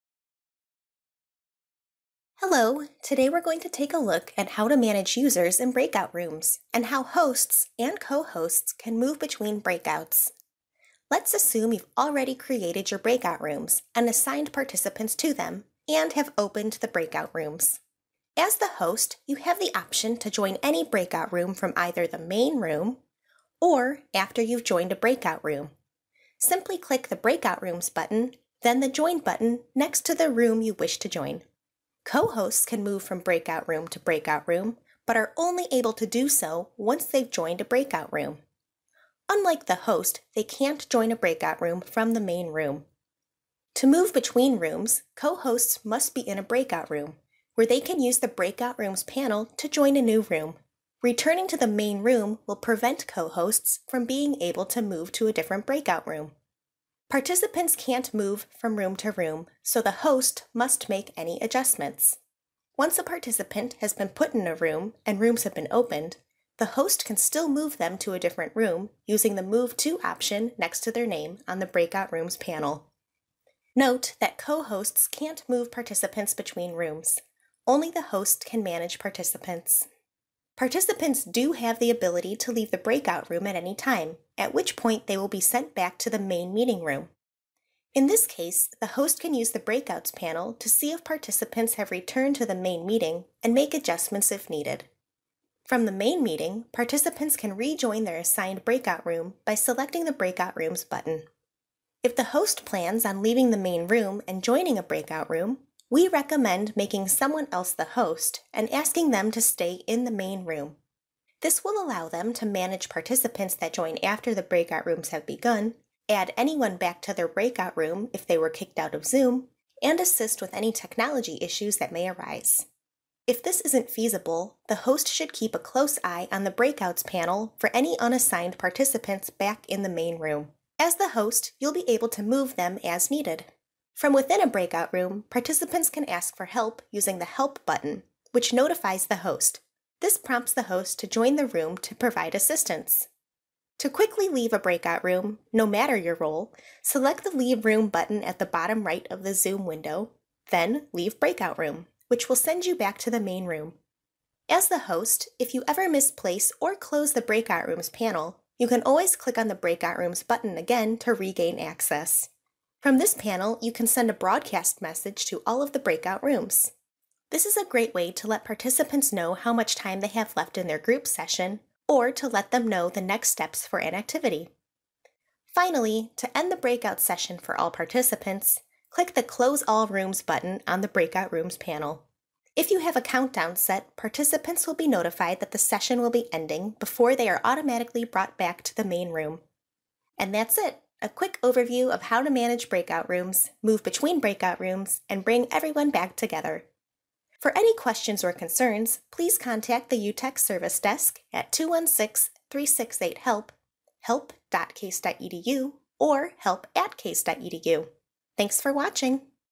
Zoom Breakout Rooms: Managing Users
This videos shows you the controls available to both hosts and co-hosts for managing participants in Zoom Breakout Rooms.

This video was made with Zoom 5.0.4. Zoom has updates frequently, so content in this video is subject to change.